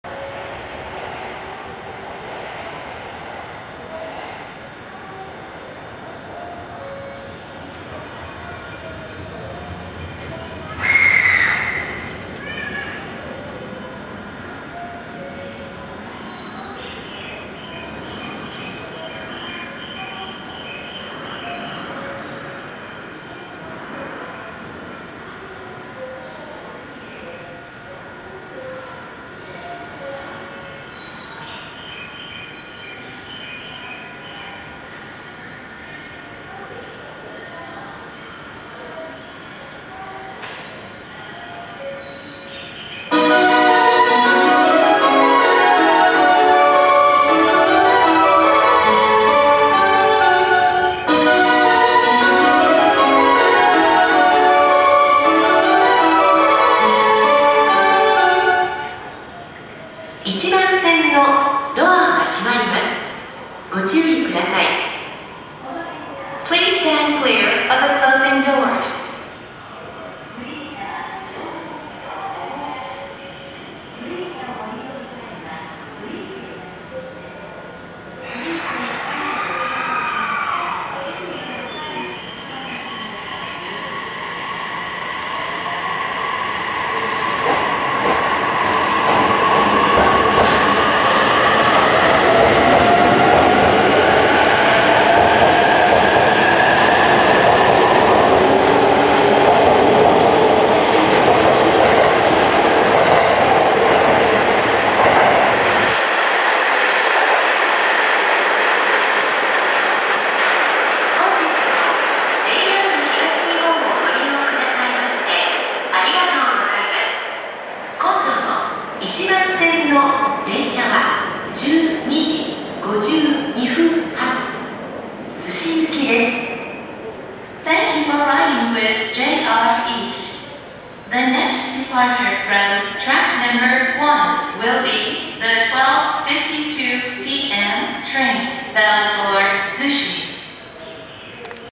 E259系 特急成田エクスプレス18号新宿行 東京発車
2020年1月26日（日）撮影
鎌倉車両センター所属E259系0番台Ne021編成
列車番号2018M（成田空港～東京間）→2218M（東京～新宿間）